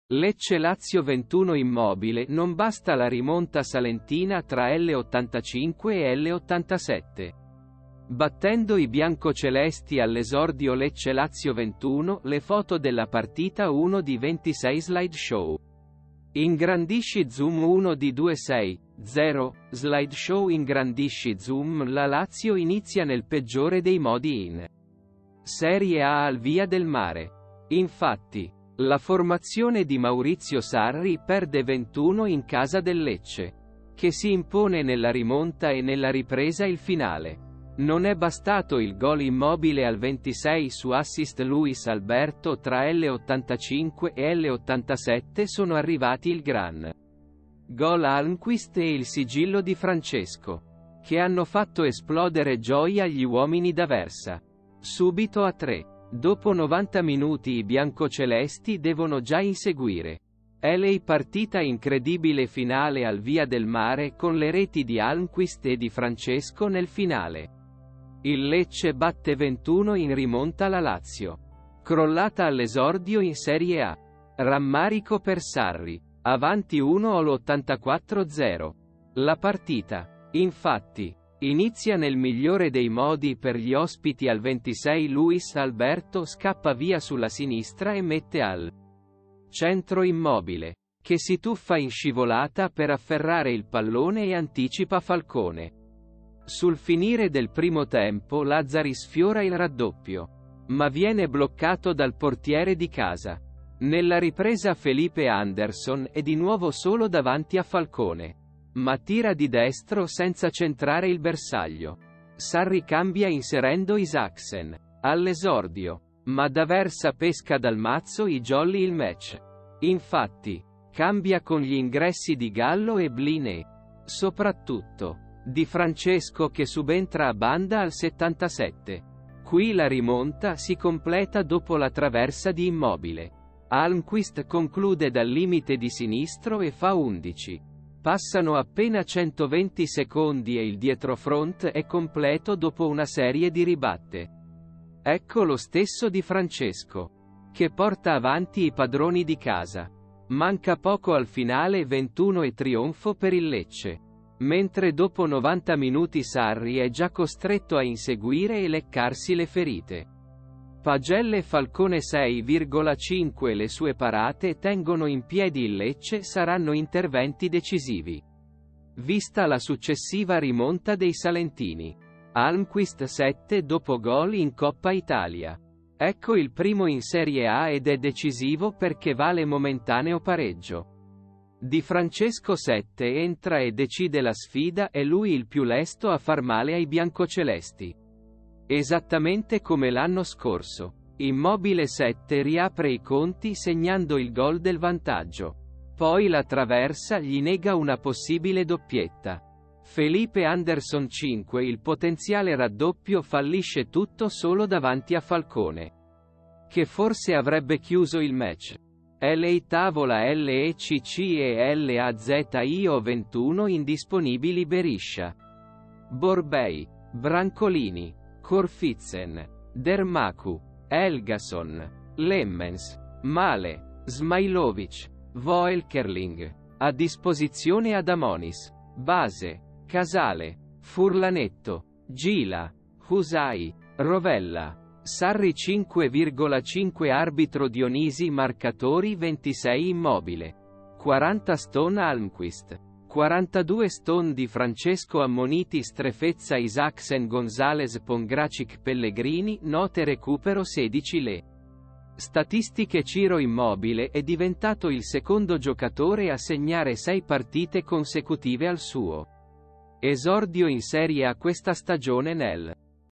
Serie A, Lecce-Lazio 2-1 Almqvist e Di Francesco ribaltano Sarri in finale
Lecce-Lazio 2-1  Immobile non basta la rimonta salentina tra l'85' e l'87', battendo i biancocelesti all'esordio Lecce-Lazio 2-1 le foto della partita 1 di 26 SLIDESHOW INGRANDISCI ZOOM 1 di 26 0 SLIDESHOW INGRANDISCI ZOOM La Lazio inizia nel peggiore dei modi in Serie A al Via del Mare, infatti, la formazione di Maurizio Sarri perde 2-1 in casa del Lecce, che si impone nella rimonta e nella ripresa il finale. Non è bastato il gol Immobile al 26' su assist Luis Alberto tra l'85' e l'87' sono arrivati ​​il ​​gran gol Almqvist e il sigillo Di Francesco, che hanno fatto esplodere gioia gli uomini D'Aversa. subito a 3. Dopo 90 minuti i biancocelesti devono già inseguire. LA PARTITA Incredibile finale al Via del Mare con le reti di Almqvist e Di Francesco nel finale, il Lecce batte 2-1 in rimonta la Lazio, crollata all'esordio in Serie A. Rammarico per Sarri, avanti 1- all'84' 0. La partita, infatti, inizia nel migliore dei modi per gli ospiti al 26' Luis Alberto scappa via sulla sinistra e mette al centro Immobile, che si tuffa in scivolata per afferrare il pallone e anticipa Falcone. Sul finire del primo tempo Lazzari sfiora il raddoppio, ma viene bloccato dal portiere di casa. Nella ripresa Felipe Anderson è di nuovo solo davanti a Falcone, ma tira di destro senza centrare il bersaglio. Sarri cambia inserendo Isaksen, all'esordio, ma D'Aversa pesca dal mazzo i jolly il match, infatti, cambia con gli ingressi di Gallo e Blin e, soprattutto, Di Francesco che subentra a Banda al 77'. . Qui la rimonta si completa dopo la traversa di Immobile, Almqvist conclude dal limite di sinistro e fa 1-1. Passano appena 120 secondi e il dietrofront è completo dopo una serie di ribatte, ecco lo stesso Di Francesco, che porta avanti i padroni di casa. Manca poco al finale 2-1 e trionfo per il Lecce, mentre dopo 90 minuti Sarri è già costretto a inseguire e leccarsi le ferite. PAGELLE Falcone 6,5 - Le sue parate tengono in piedi il Lecce saranno interventi decisivi, vista la successiva rimonta dei salentini. Almqvist 7 - Dopo gol in Coppa Italia, ecco il primo in Serie A ed è decisivo perché vale momentaneo pareggio. Di Francesco 7- Entra e decide la sfida è lui il più lesto a far male ai biancocelesti, esattamente come l'anno scorso. Immobile 7 - Riapre i conti segnando il gol del vantaggio, poi la traversa gli nega una possibile doppietta. Felipe Anderson 5 - Il potenziale raddoppio fallisce tutto solo davanti a Falcone, che forse avrebbe chiuso il match. LA TAVOLA LECCE-LAZIO 2-1 Indisponibili Berisha, Borbei, Brancolini, Corfitzen, Dermaku, Helgason, Lemmens, Maleh, Smajlovic, Voelkerling. A disposizione Adamonis, Base, Casale, Furlanetto, Gila, Hysaj, Rovella. Sarri 5,5 Arbitro Dionisi Marcatori 26' Immobile , 40' st Almqvist , 42' st Di Francesco Ammoniti Strefezza Isaksen Gonzalez Pongracic Pellegrini Note recupero 1'+6' LE STATISTICHE Ciro Immobile è diventato il secondo giocatore a segnare sei partite consecutive al suo esordio in Serie A questa stagione nell'era dei tre punti a vittoria , dopo Luis Muriel . Ciro Immobile ha raggiunto quota 200 gol nei cinque grandi campionati europei. Inoltre, questo è stato il centesimo gol in trasferta di Immobile nei cinque grandi campionati europei. Per la prima volta il Lecce vince l'esordio stagionale in Serie A, prima di oggi i pugliesi avevano ottenuto sei pareggi e 11 sconfitte in questa sfida di campionato. Luis Alberto ha servito sette assist nel 2023 in Serie A, tra i centrocampisti nei top-5 campionati europei nessuno ha fatto meglio dello spagnolo. Pontus Almqvist è il secondo svedese a segnare con il Lecce in Serie A dopo Klas Ingesson . Rares Burnete è il secondo giocatore più giovane del Lecce ad aver servito un assist in Serie A dopo Valeri Bozhinov da quando Opta ha raccolto i dati . Solo contro l'Atalanta Federico Di Francesco ha segnato più gol che contro la Lazio in Serie A. È passato 1 minuto e 35 secondi dalle reti di Pontus Almqvist e di Federico Di Francesco . Patrick Dorgu è il più giovane giocatore del Lecce a scendere in campo tra questa e la scorsa stagione.